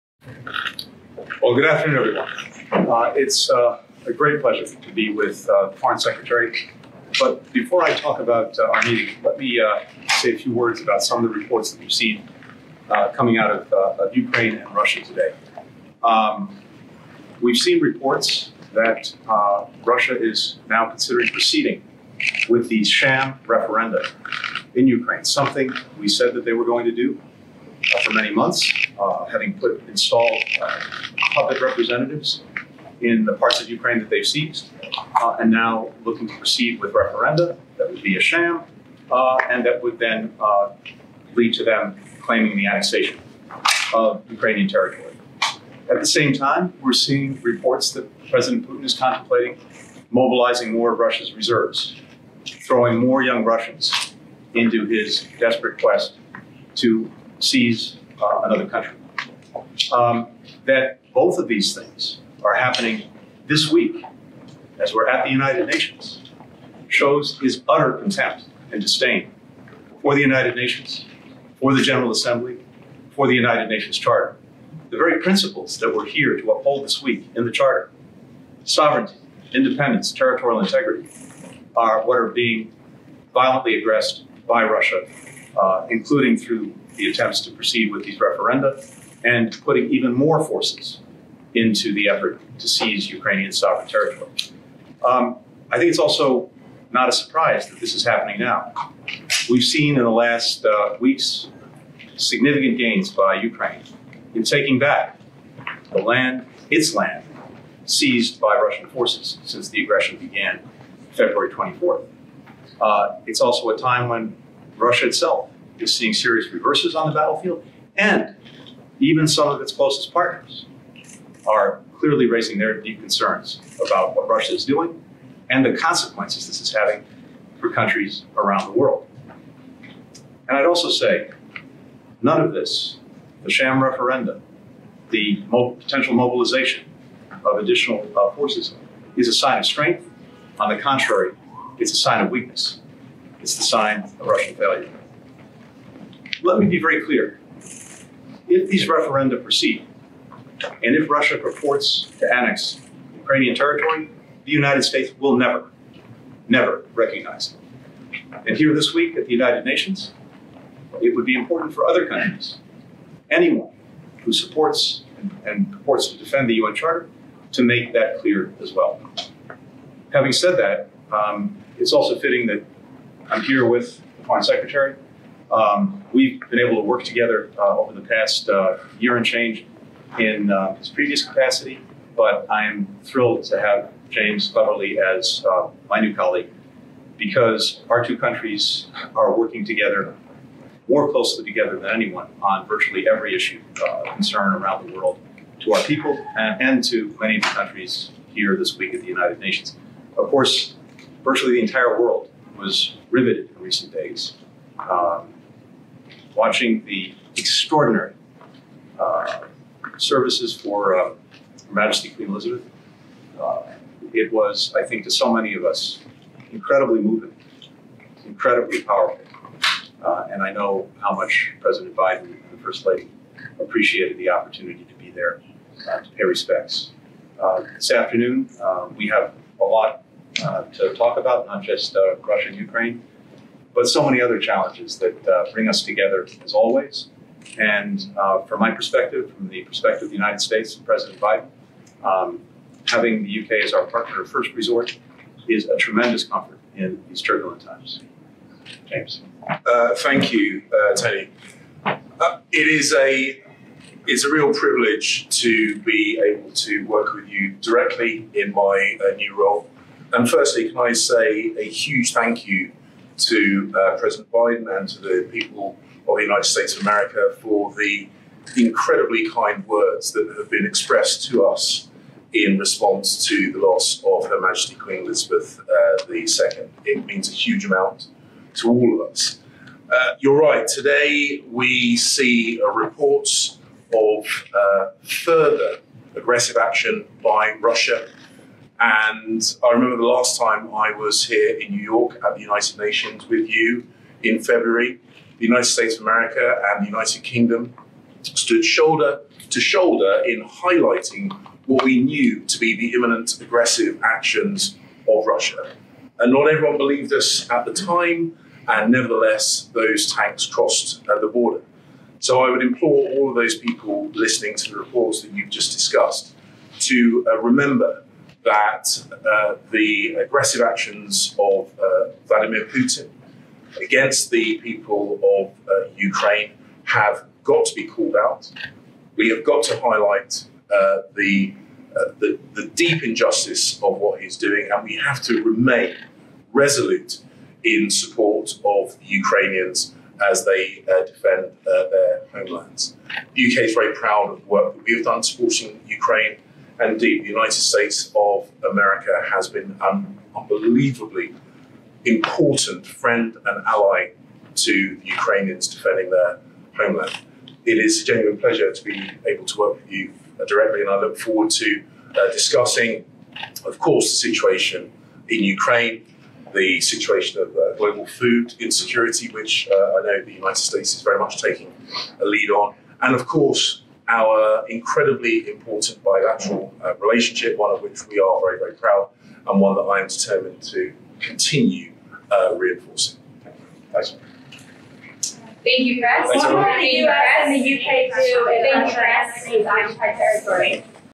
Secretary Blinken meets with UK Foreign Secretary James Cleverly in New York City, New York.
Secretary Blinken meets with UK Foreign Secretary James Cleverly in New York City, New York, on September 20, 2022.